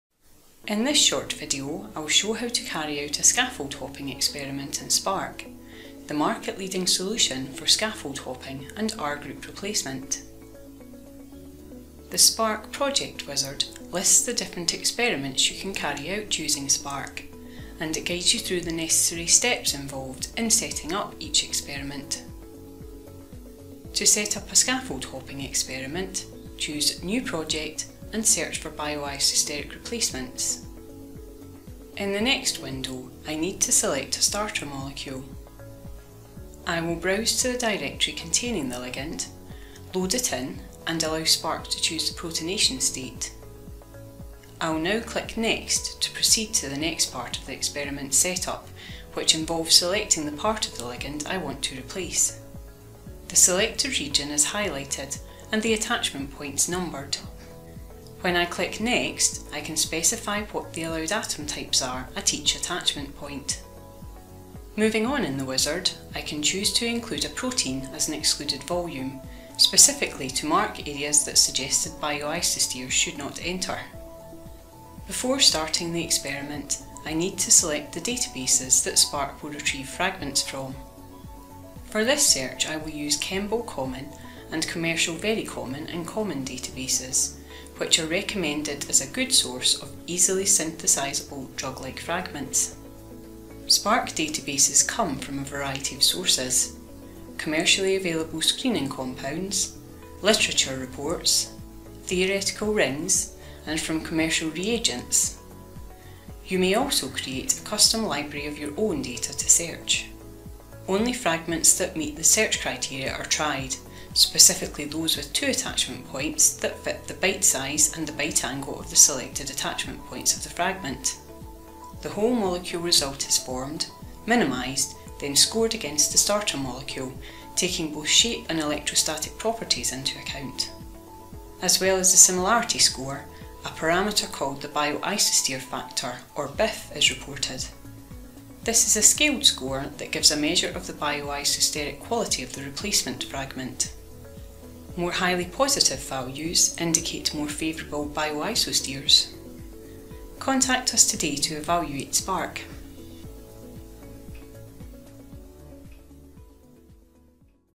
Introduction to scaffold hopping using Spark™
Learn how to carry out a scaffold hopping experiment in Spark, the leading solution for scaffold hopping and R-group replacement.